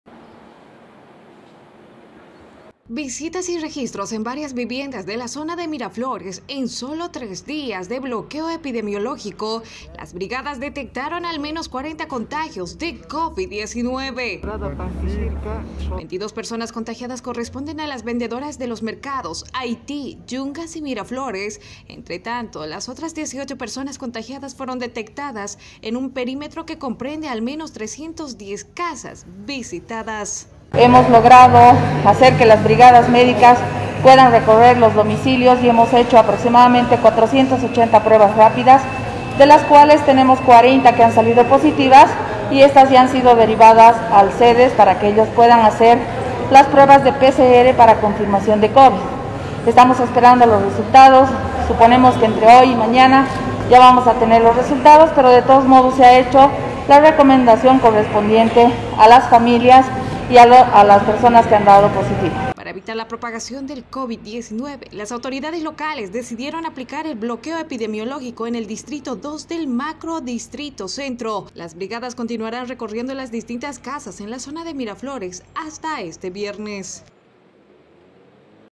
Detectan 40 casos positivos en Miraflores
Tras que el pasado viernes iniciara el bloqueo epidemiológico en una zona de la ciudad de la Paz, varios grupos médicos se desplazaron por diferentes viviendas de Miraflores los cuales hasta este domingo pudieron detectar a 40 personas que dieron positivo en los test que realizaron.